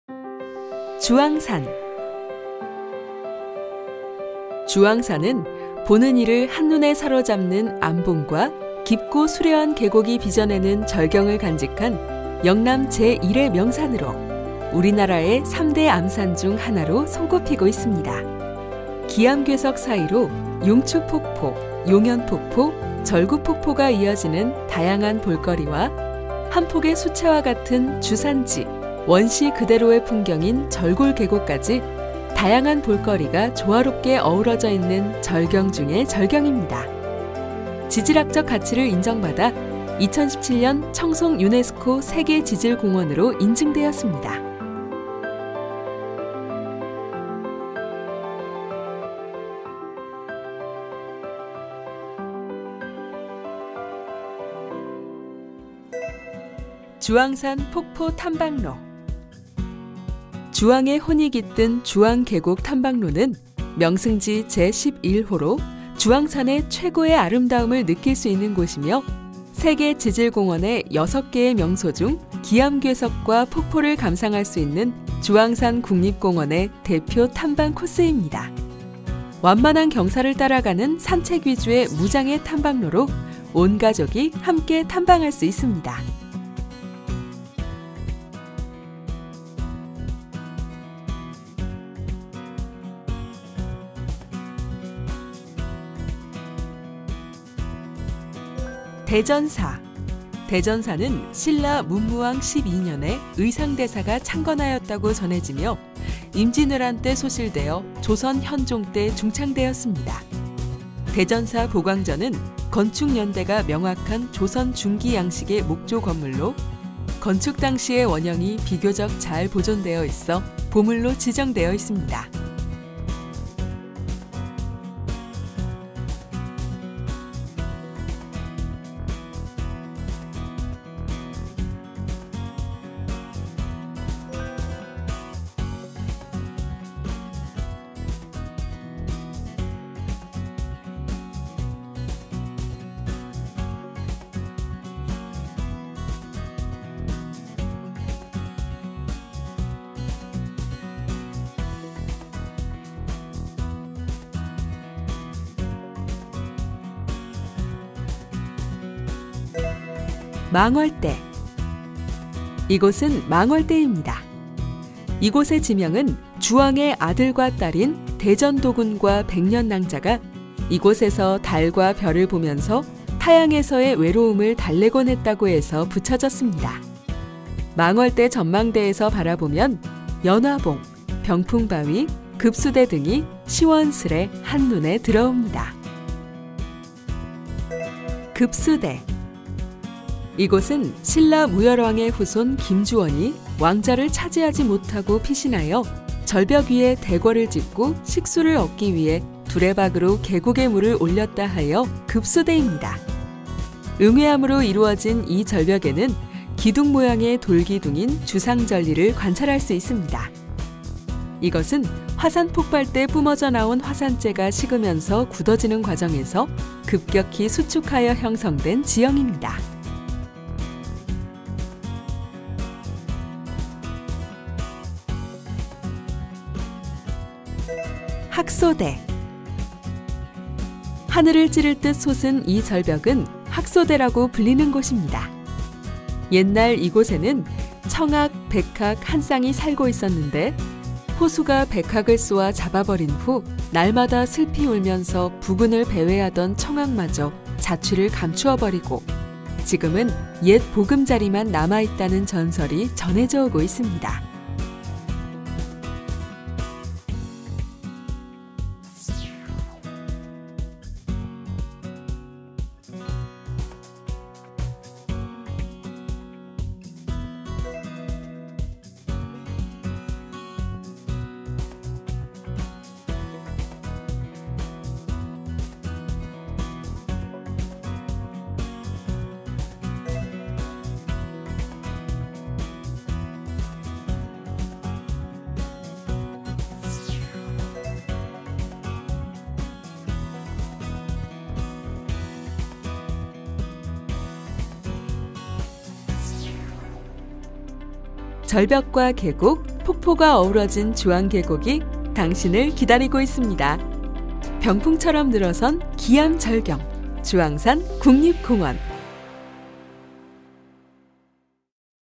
[4K] VR(360도 영상)로 즐기는 주왕산국립공원 가상탐방 / 주왕계곡 / Juwangsan National Park /
VR(360도 영상)로 즐기는 주왕산국립공원 가상탐방 / 주왕계곡  / Juwangsan National Park  /

주왕산 국립공원은 1976년 우리나라 12번째 국립공원으로 지정되었으며 암봉과 깊고 수려한 계곡이 빚어내는 절경을 간직한 영남 제1의 명승지이다.

태행산(933.1m), 대둔산(905m), 명동재(875m), 왕거암(907.4m) 등의 산들이 말발굽형으로 자연성곽 같은 멋진 산세를 이루고 있으며, 7천만 년 전의 용암이 흘러내리면서 굳은 용결 응회암으로 이루어져 특색있는 경관을 이루고 있어 우리나라의 3대 암산 중 하나로 손꼽히고 있다.

이제 집에서도 주왕산국립공원을 만나보세요.

Virtual tour of Juwangsan National Park

Discover the beautiful mountain and stream of the Juwangsan National Park

Juwangsan National Park was designated as the 12th national park in Korea in 1976. It borders Cheongsong-gun and Yeongdeok-gun in Gyeongsangbuk-do.
There are 88 plant species including endangered species such as the Nodding lily, Tattaasau and Yellow water flag in Juwangsan National Park. As for animals, 924 animal species inhabit the park area and among them the Otter has been designated as a Natural Treasure.